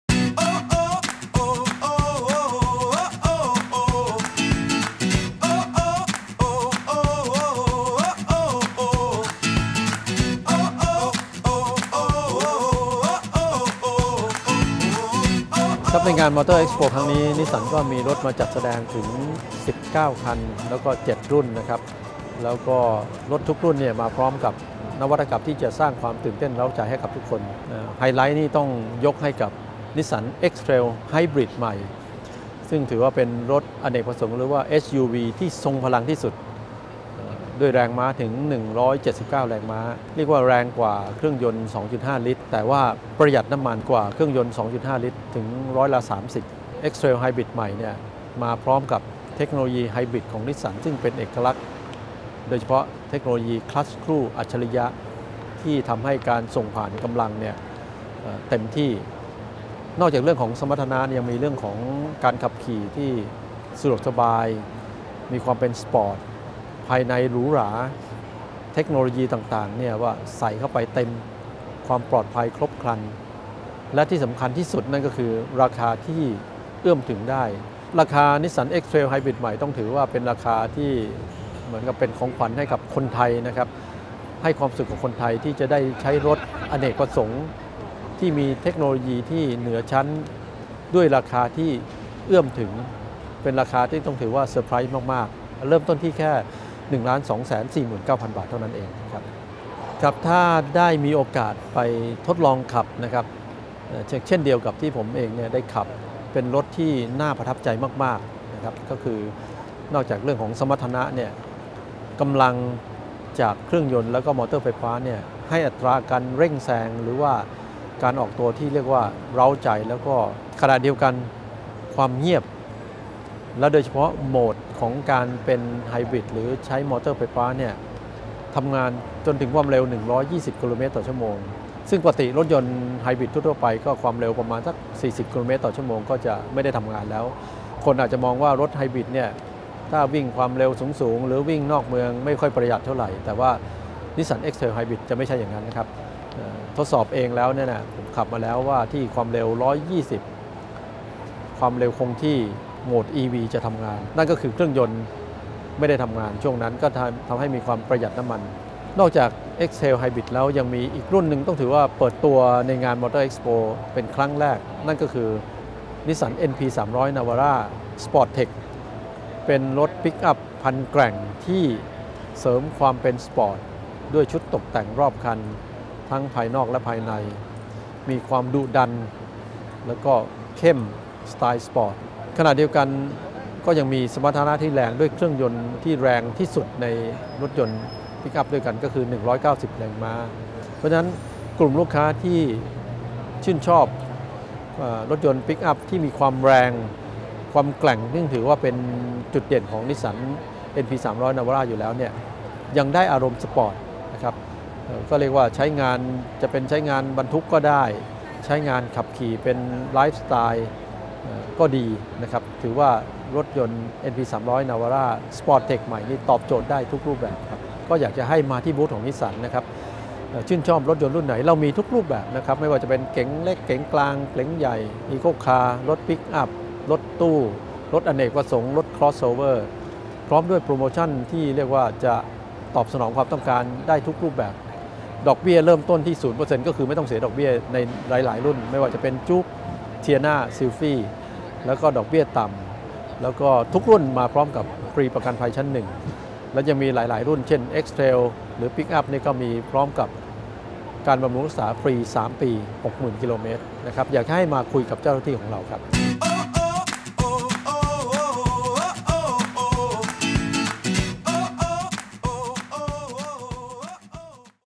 "นิสสัน" มอบของขวัญปีใหม่คนไทย จัดโปรฯแรงที่สุด พร้อมแนะนำ X-Trail Hybrid ในราคาที่ไขว่คว้าได้
พาไปคุยกับ "ประพัฒน์ เชยชม" รองกรรมการผู้จัดการใหญ่อาวุโส ฝ่ายตลาดและขาย นิสสัน มอเตอร์ (ประเทศไทย) ถึงภาพรวมบูธนิสสัน ในงานมหกรรมยานยนต์ ครั้งที่ 32 หรือ Motor Expo 2015 ระบุว่า ครั้งนี้จัดเต็มโปรโมชั่น เพื่อเป็นของขวัญคนไทย และยังแนะนำรถยนต์ไฮบริดรุ่นแรกของนิสสัน Nissan X-Trail Hybrid รถไฮบริดในราคาที่จับต้องได้ เพื่อคนไทยได้ใช้รถยนต์เทคโนโลยีสูง ในราคาคุ้มค่า และจริงใจ